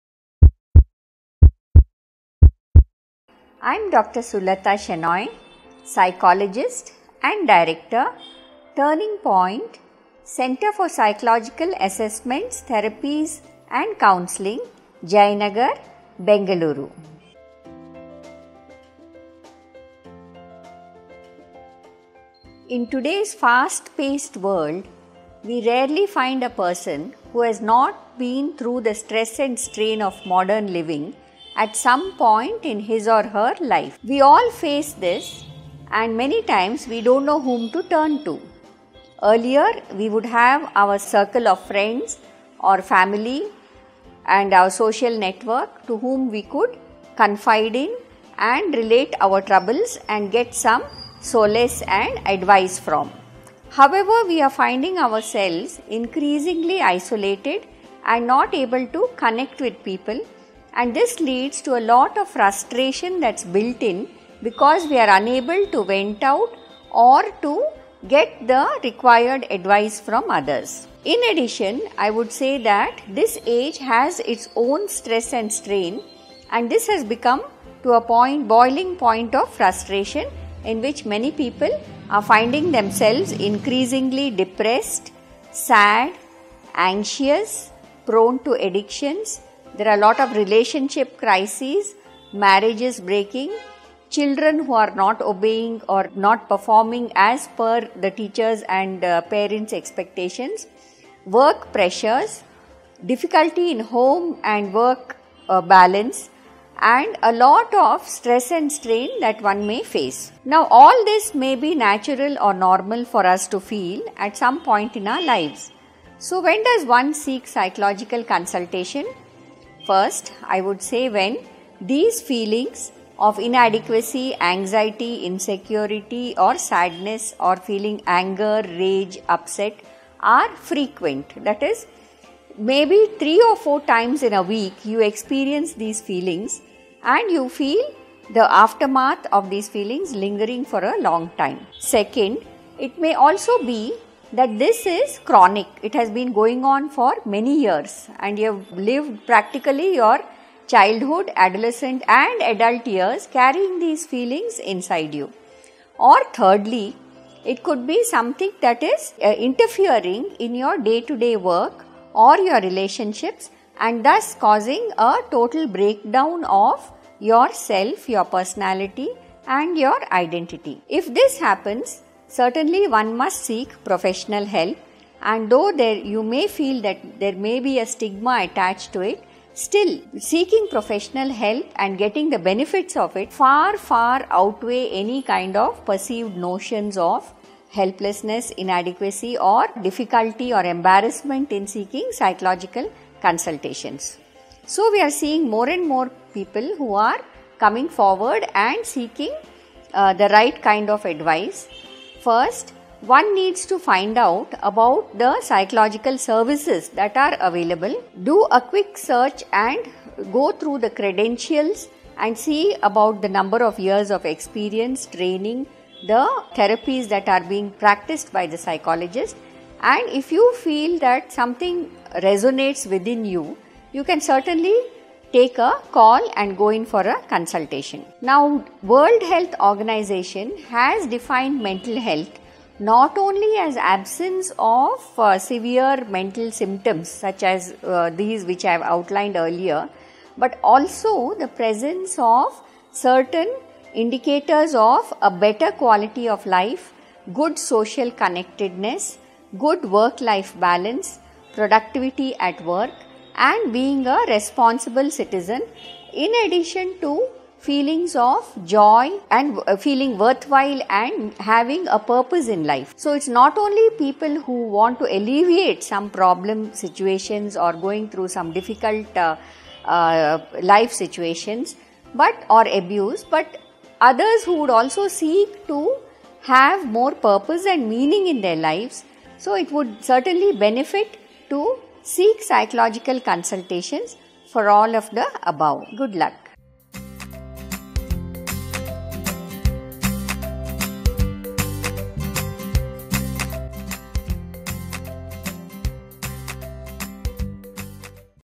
Who would benefit from a psychological consultation? - Dr. Sulata Shenoy
In today’s fast life we rarely see a person who has not been through the stress and strain of modern living at some point of his or her life. We all faced this and many times we don’t know who to turn to. Earlier we have our circle of friends or family and social network to whom we could confide it and relate our troubles and get some solace and advice. We are finding ourselves increasingly isolated and not able to connect with people and this leads to a lot of frustrations that is built in because we are unable to bring it out or to get the required advice from others. Many people are finding themselves increasingly sad, depressed, anxious, prone to addictions and there are lot of relationship crisis, falling marriages, children who are not obeying or not performing as per the teachers or parents expectations, work pressure, difficulty in work and home balance and a lot of stress and strain that one may face. All this may be natural or normal for us to feel at one point of life. One needs psychological consultation when these feelings of inadequacy, anxiety, insecurity or sadness or feeling anger, greed, upset are frequent, i.e. maybe three or four times in a week and you feel the after mark of these feelings lingering for a long time. Second is it may also be that this is chronic and been going on for many years and you live practically your childhood, adolescent and adult life carrying these feelings inside you or it could be something athat could be interfering your day to day work all your relationships causing total breakdown of your self, your personality and your identity. if this happens certainly one must seek professional help. There is various stigma attached to it. Seeking professional help and getting benefits of it far outweigh any kind of perceived notions of helplessness, inadequacy, difficulty or embarrassment in seeking psychological consultations. So we are seeing more and more people who are coming forward and seeking the right kind of advice. First one needs to find out about the psychological services that are available, do a quck search and go for the credentials and see about the number of years of experience, the therapies that are practiced by the psychologist and if you feel that something resonates within you, you can certainly take a call and go in for a consultation. World Health Organisation has defined has defined mental health not only as absence of severe mental symptoms but also the presence of certain indicators of a better quality of life, good social connectiveness, good work life balance, productivity at work and being a responsible in addition to feelings of joy and feeling worthwhile and having a purpose in life. So it is not only people who want to alleviate some problems and situations but others who would also seek to have more purpose and meaning in their lives.